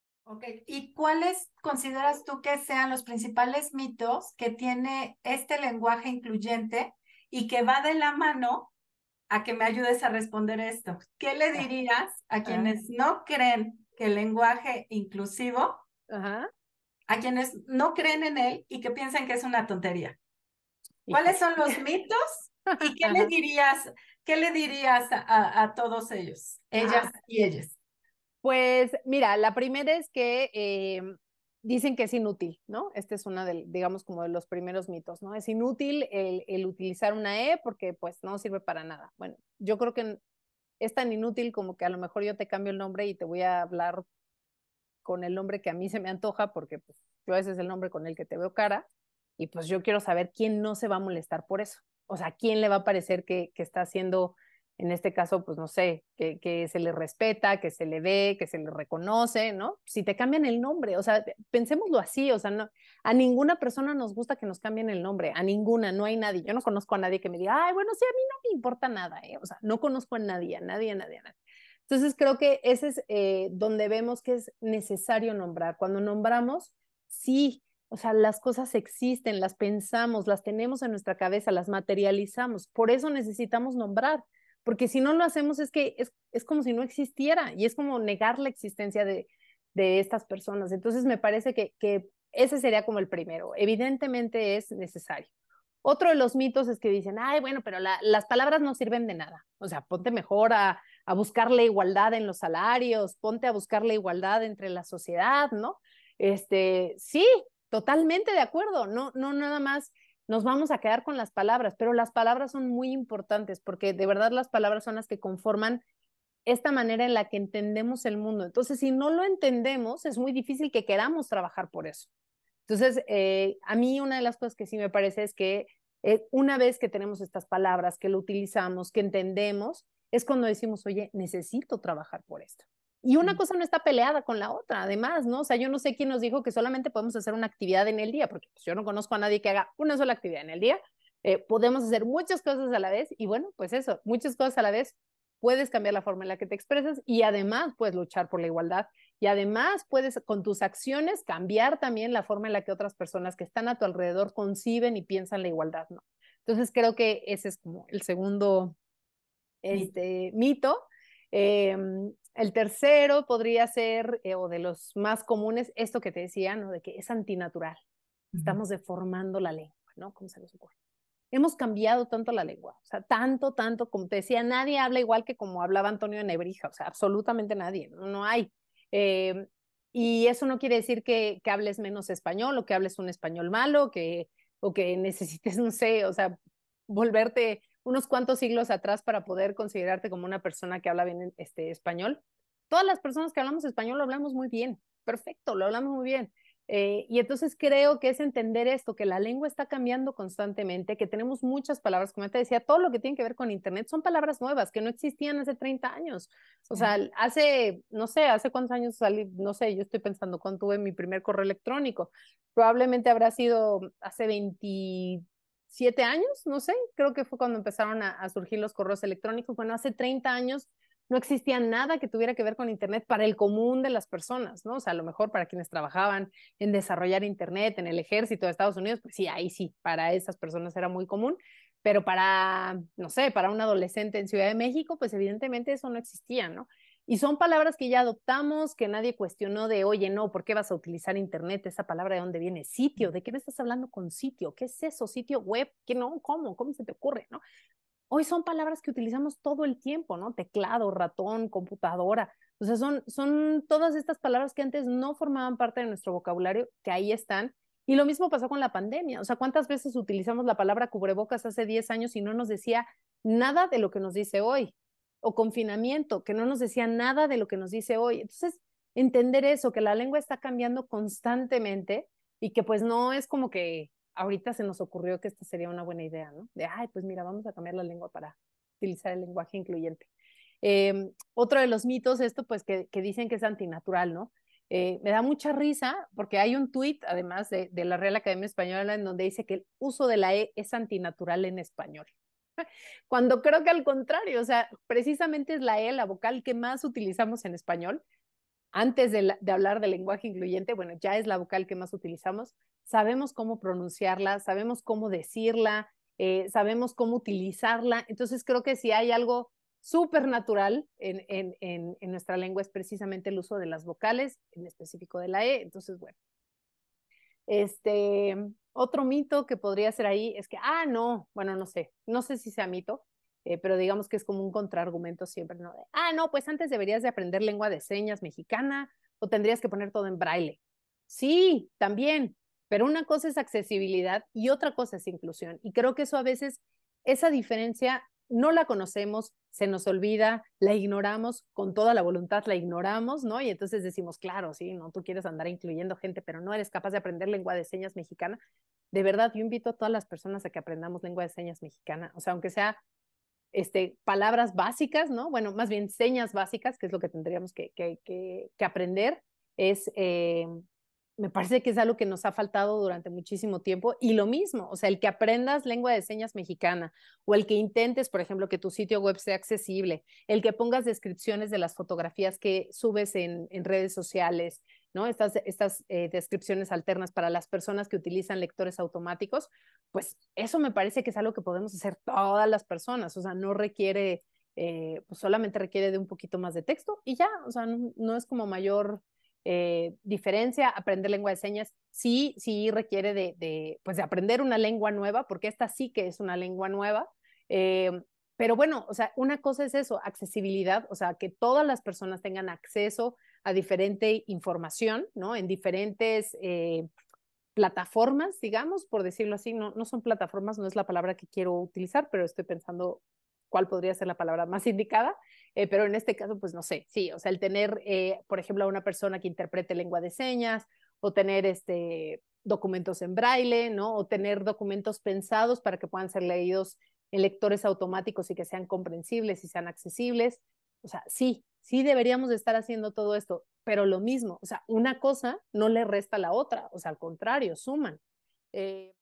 Mensaje para quiénes no creen en el lenguaje inclusivo
Fragmento de la plática con Paulina Chavira sobre lenguaje inclusivo.